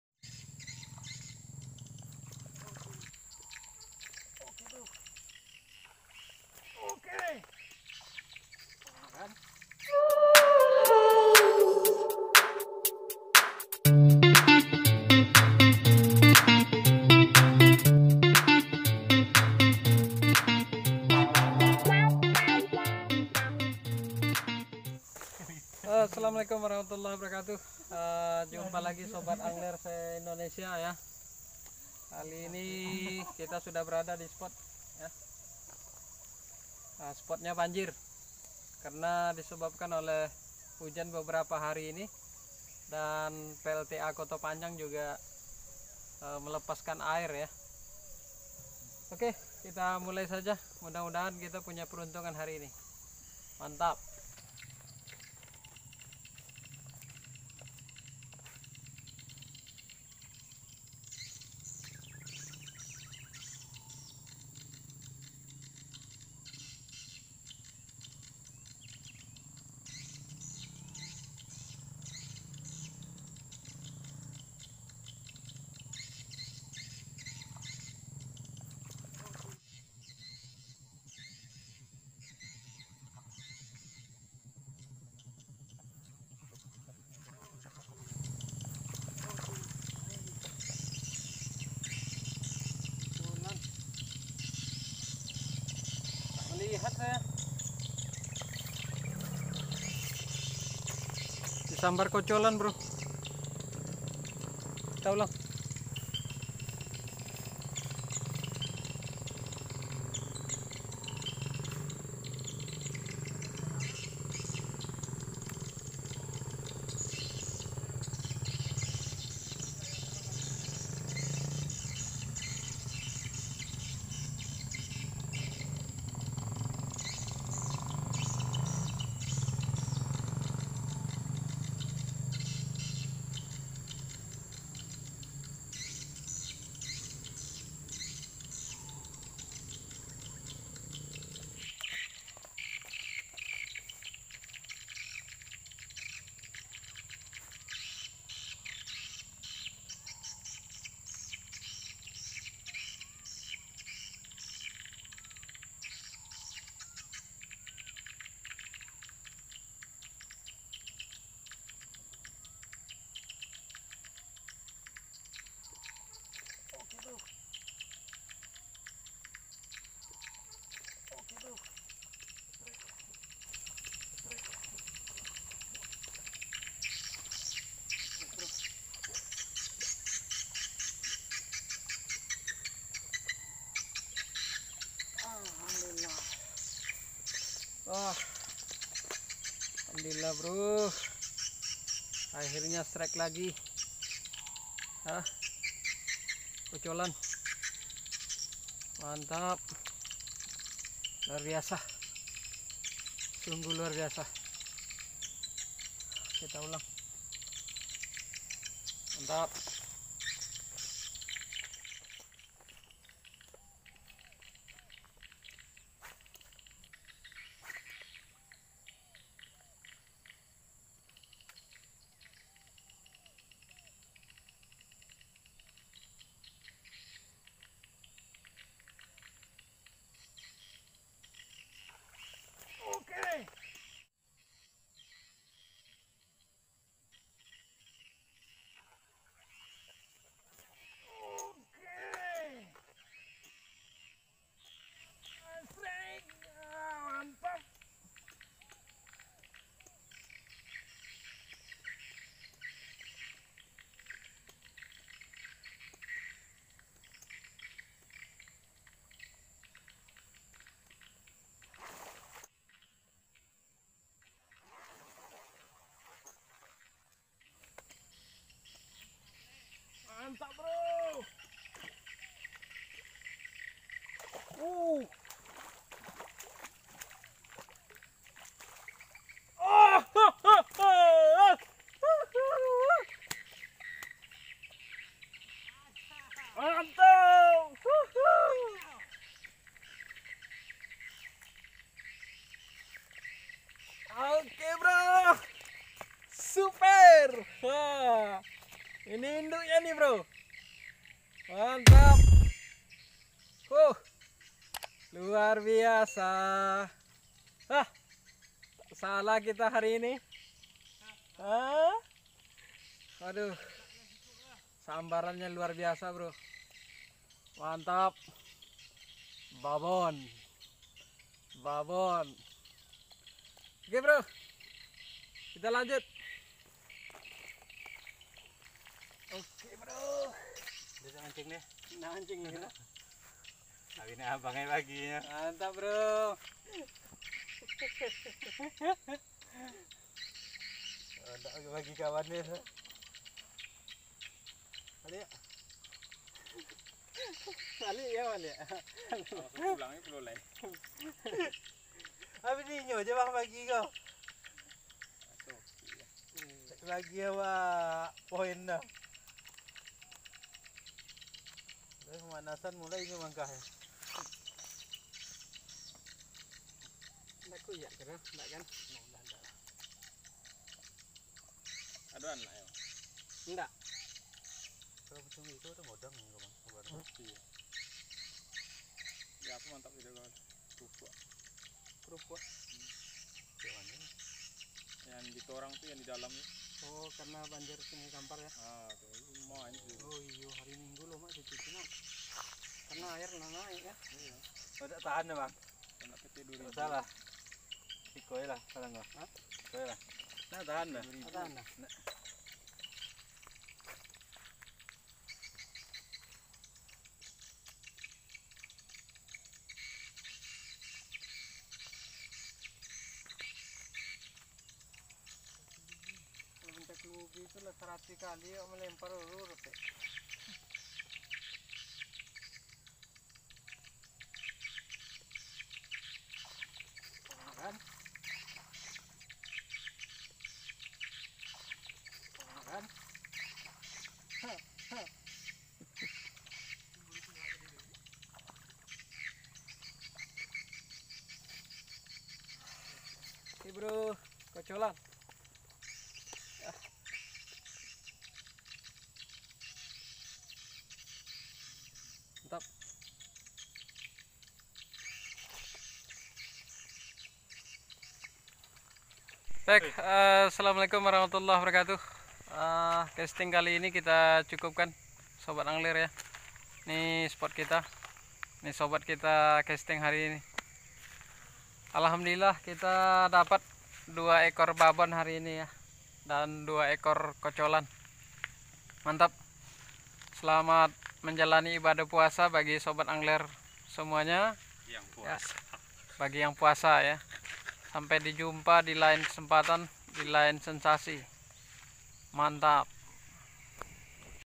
Ganasnya Sambaran Haruan Besar di Kondisi Spot Banjir || Casting Snakehead
Assalamualaikum sobat Angler semua..
Selamat menjalani ibadah puasa...
Coba2 peruntungan di spot lagi banjir yang disebabkan oleh hujan dalam beberapa hari dan ditambah PLTA Koto Panjang membuka pintu air, sehingga sungai Kampar meluap kemana2...
Spot ini berjarak lebih kurang 45 KM dari Pekanbaru..
Alhamdulillah Sambaran babon haruan membuatku senang dan candupun kesampaian...😀

Trims atas dukungan sobat semua atas channel ini..